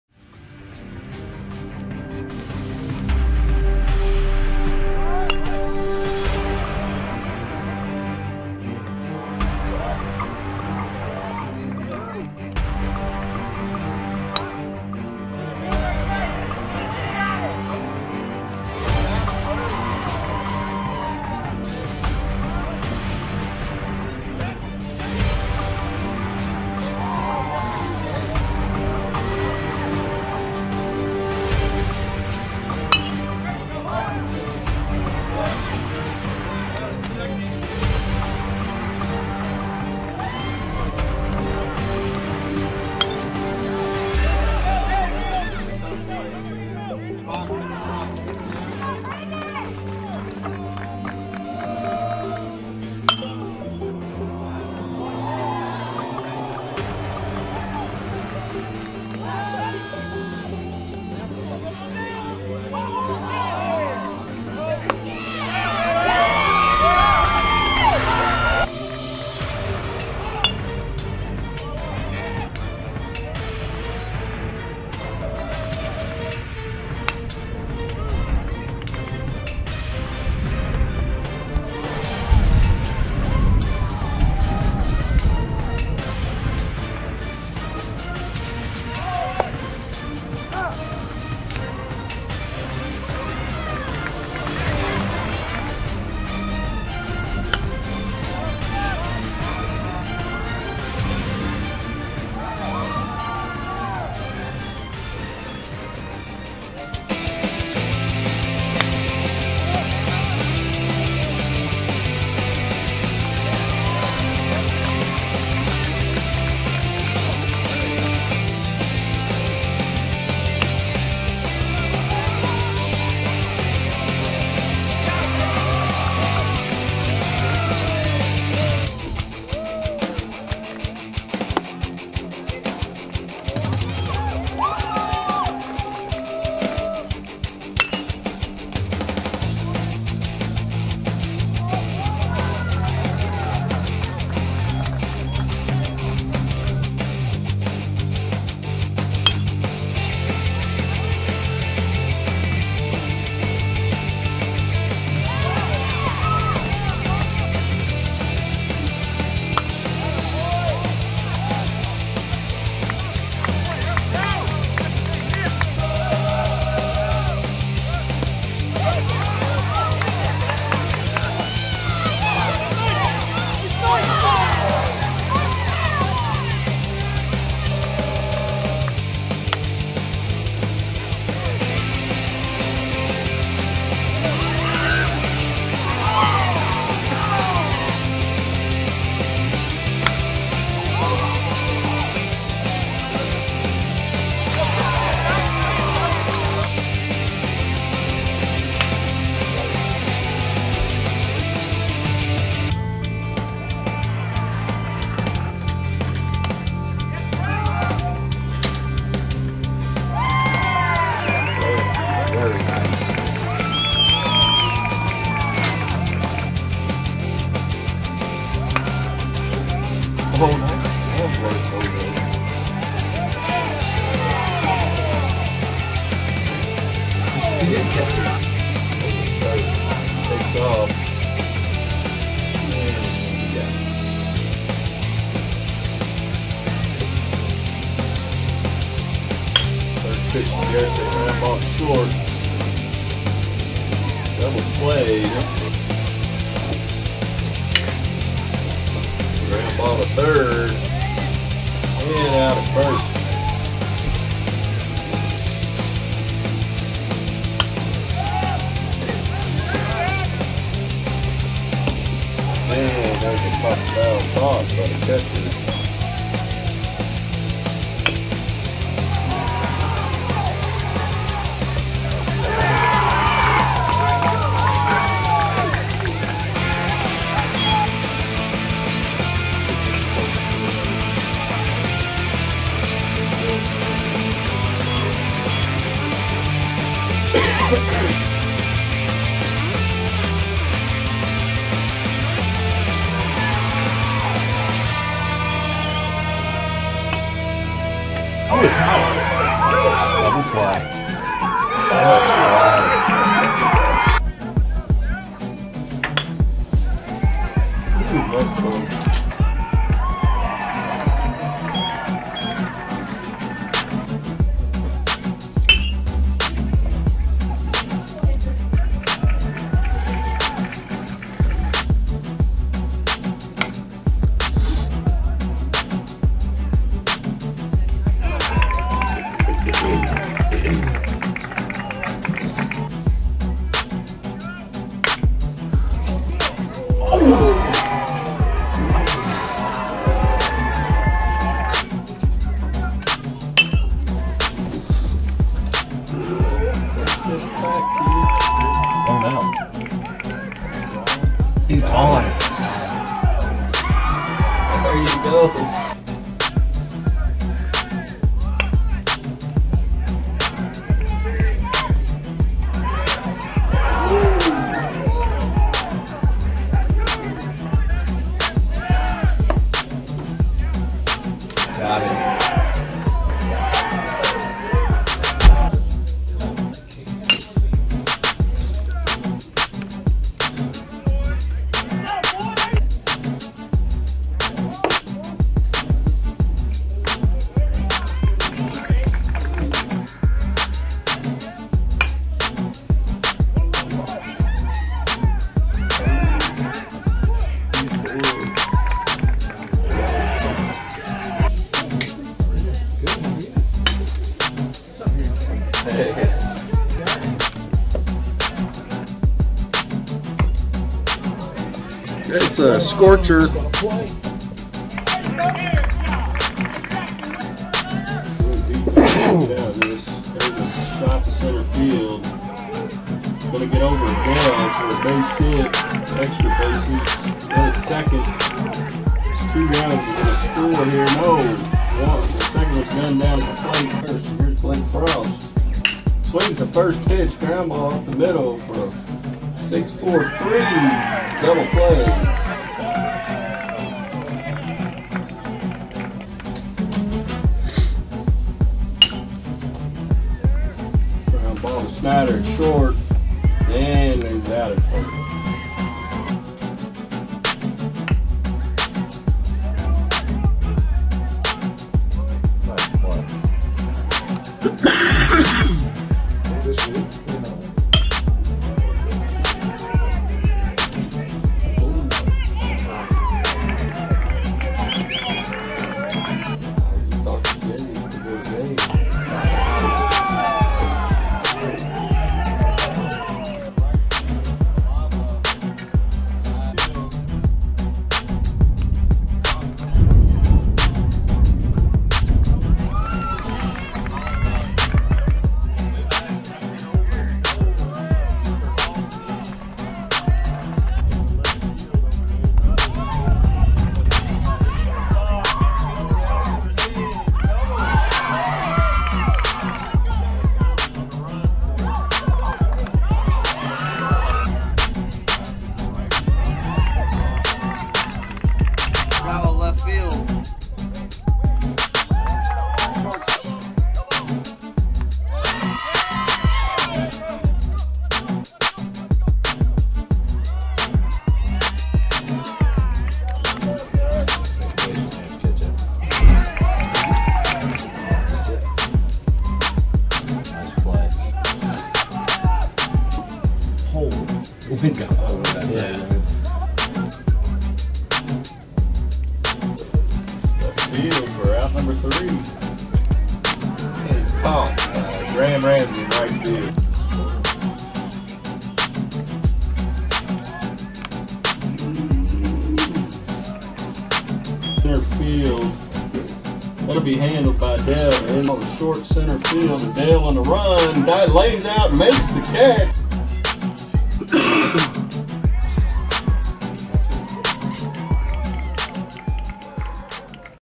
Best of Defense at 2016 KY Super Prep
Some of the best Defense recorded at 2016 KY Super Prep Baseball on the new Turf Field at Ballard H.S. in Louisville, KY.